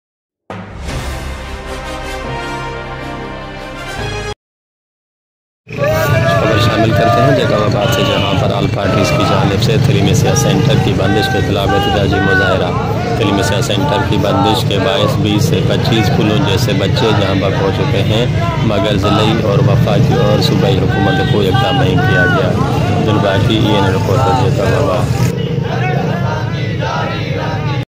enn tv : abdul baqi news
جیکب آباد : احتجاج دھرنا روڈ بلاک

 (  نمائندہ عبدالباقی ۔  سٹی رپورٹر ای این این )

جیکب آباد تھیلیسیمیا سینٹر کی بحالی کے لئے آل پارٹیز کمیٹی کا شعبان ابڑو محمد حسن ٹالانی شاہد جوادی ظہور مائری کی قیادت میں احتجاج دھرنا روڈ بلاک دو گھنٹے تک ٹریفک معطل اس موقع پر  رہنماؤں نے خطاب کرتے ہوئے کہا کہ جان بوج کر تھیلیسیمیا سینٹر کو بند کر کے معصوم بچوں کی زندگیاں چھین لی ہیں

 20سے 25 بچے جانبحق ہوگئے

 مگر انتظامیہ کوئی اقدام نہیں کر رہی سات ماہ سے سینٹر بند ہے انہوں نے مزید کہا کہ وفاقی وزیر محمد میاں سومرو کچھ دن قبل ڈی سی آفس میں منعقد اجلاس میں کہا تھا کہ تھیلیسیمیا سینٹر کو بحال کیا جائے گا

 مگر افسوس اب تک کوئی عمل نہیں ہو سکا  خون نہ ملنے کے باعث پہولوں جیسے معثوم بچے جانبحق ہورہےہیں

 مظاہرین نے وزیرِ اعلیٰ سندھ سید مراد علی شاہ صوبائی مشیر اعجاز جاکہرانی سے مطالبہ کرتے ہوئے کہا کہ تہیلیمیسیا سینٹر کو جلد از جلد بحال کروائیں تاکہ معثوم بچے زندہ رہ سکیں

دہرنے میں عام انسان تحریک کے مرکزی رہنما عید محمد ڈومکی عوامی اتحاد چیئرمین عزیز سیال تاج محمد دایو جے یو پی نورانی کے رفیق فخری نیشنل پارٹی کے علامہ شاہد جوادی شیعہ امن کمیٹی مرتضیٰ انقلابی سندھ ملاح فورم کے الطاف میرانی سولنگی اتحاد کے عمران سولنگی ڈومکی ویلفیئر کے لیاقت ٹالانی مسلم لیگ ق کے صابر حسین نے شرکتِ کی